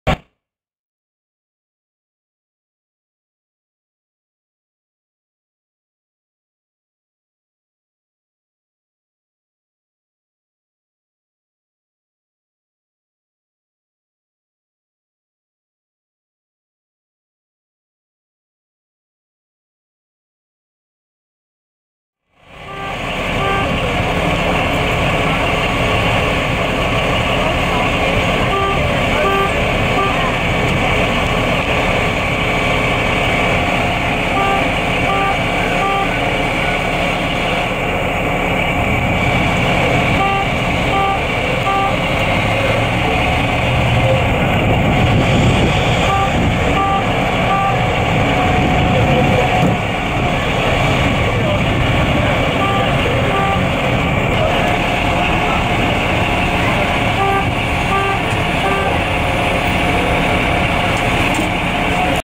Accident at Family Video in Farmington
This car slammed into the Family Video store in Farmington on Monday afternoon after the driver reportedly suffered a medical problem and lost control of the vehicle.